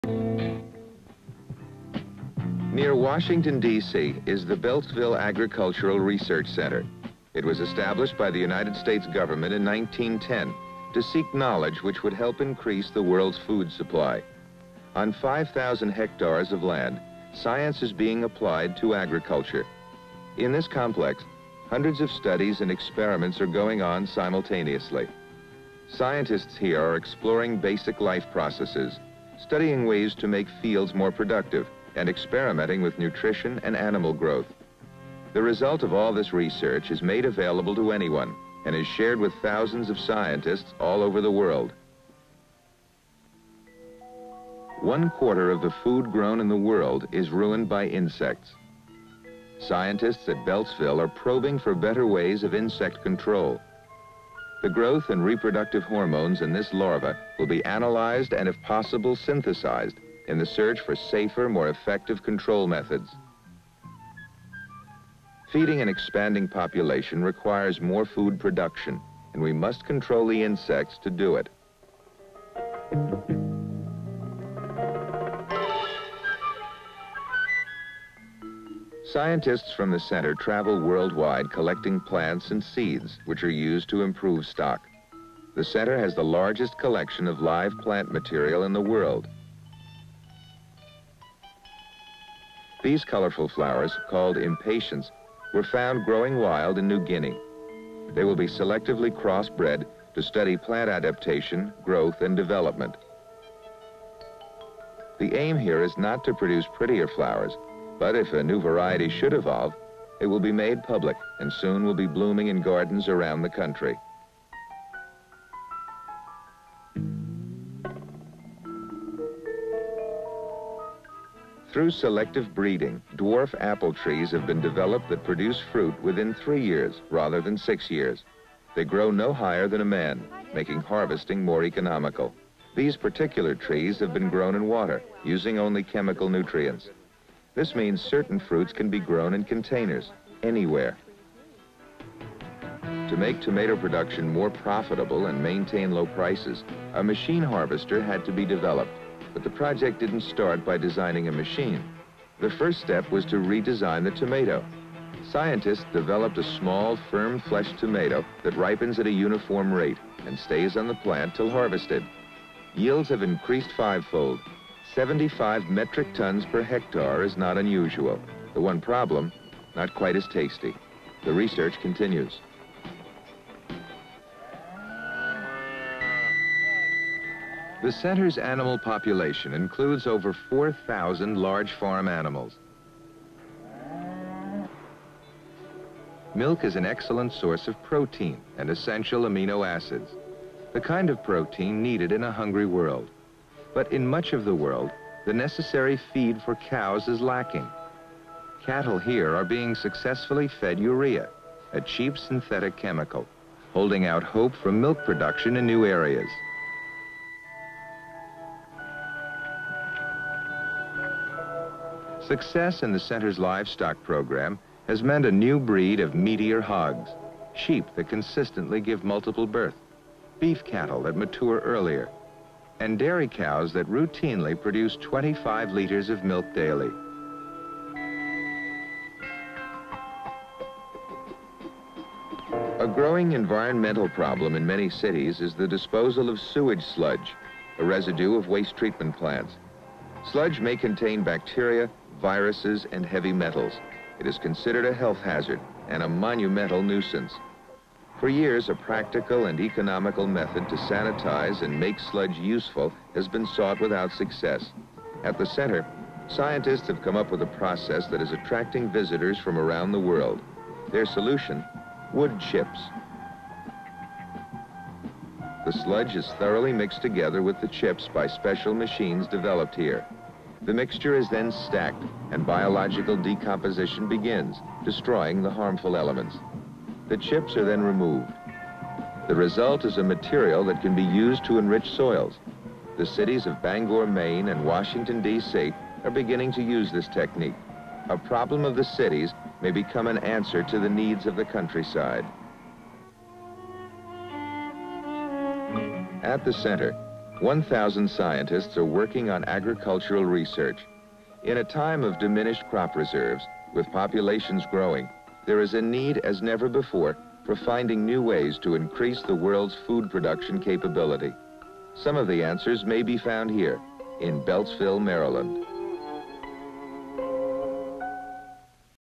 FOOD PRODUCTION RESEARCH -1977
BELTSVILLE:
PAN BUILDING COMPLEX, CAMPUS, MAKING UP BELTSVILLE
AGRICULTURAL RESEARCH CENTER, ON 5000 ACRES OF
LAND NEAR WASHINGTON, D.C. ESTABLISHED BY U.S.
GOVERNMENT IN 1910 TO HELP INCREASE WORLD'S FOOD
SUPPLY.

00:46:40
PAN ZOOM IN ON MAL, FEMALE, SCIENTISTS WORKING IN
CORN FIELD AT CENTER TO MAKE IT MORE PRODUCTIVE.

00:46:59
CU FEMALE SCIENTIST DISSECTS GREEN WORM-LIKE
LARVAE, PLACES IT UNDER MICROSCOPE. ONE-QUARTER
OF FOOD GROWN IN WORLD IS RUINED BY INSECTS. CU
SCIENTISTS HANDS SEPARATING LARVAE WITH TWEEZERS.

00:47:39
CU MALE SCIENTIST LIFTS POLLEN FROM CENTER OF
FLOWER, CALLED IMPATIENT, WITH SMALL BRUSH. FOUND
IN NEW GUINEA, THEY WILL BE SELECTIVELY CROSS-BRED
TO STUDY PLANT ADAPTATION, GROWTH, DEVELOPMENT.
AIM IS TO PROVIDE PUBLIC, NEW VARIETY OF FLOWER.

00:48:15
WORKERS WATER DWARFS APPLE TREES, WHICH PRODUCE
FRUIT WITHIN THREE YEARS, GROWING NO HIGHER THAN
MAN, MAKING HARVESTING MORE ECONOMICAL.

00:48:21
SCIENTIST WATERS FRUIT TREE GROWN IN POT OF
WATER, USING ONLY CHEMICAL NUTRIENTS. THIS MEANS
CERTAIN FRUITS CAN BE GROWN IN CONTAINERS,
ANYWHERE. PAN OF ROWS OF TOMATO PLANTS FULL WITH
FRUIT. HARVESTING MACHINE SCOOPS UP TOMATOES,
CONVEYS THEM ONTO TABLE, WHERE THEY ARE SEPARATED,
PLACED ON CONVEYOR, BOXED.

00:49:12
CU MILKING MACHINE ATTACHED TO COW. SCIENTIST
MONITORS LARGE GALLONS OF MILK BEING PRODUCED BY
MILKING, IN EFFORT TO INCREASE WORLD PRODUCTION.
CATTLE HERE ARE SUCCESSFULLY FED UREA, CHEAP
SYNTHETIC CHEMICAL, HOLDING OUT HOPE FOR MILK
PRODUCTION IN NEW AREAS.

00:49:41
SCIENTIST MEASURES HEIGHT, LENGTH OF COW.

00:50:10
DUMP TRUCK CARRIES LOAD OF SEWERAGE SLUDGE TO YARD
CONTAINING HUGE PILES OF THE SLUDGE, DUMPS LOAD.
SCIENTIST AT CENTER HAVE COME UP WITH METHOD OF
MAKING USE OF THE SLUDGE BY COMBINING IT WITH WOOD
CHIPS, WHICH DESTROY HARMFUL ELEMENTS DURING
DECOMPOSITION.

00:50:24
SCIENTIST FORM ALL OVER WORLD WATCH SPECIAL
MACHINERY, BULLDOZERS, PILE SLUDGE, WOOD CHIPS,
STACKED FOR BIOLOGICAL DECOMPOSITION TO BEGIN.
CHIPS ARE REMOVED. RESULT IS MATERIAL THAT CAN BE
USED TO ENRICH SOIL.

00:51:08
CU SCIENTIST USING TEST TUBES IN LAB AT CENTER
WHERE ONE THOUSAND SCIENTISTS ARE WORKING ON
AGRICULTURAL RESEARCH. CU FACE SHOT OF SCIENTIST
WATCHING TEST TUBE BEING FILLED.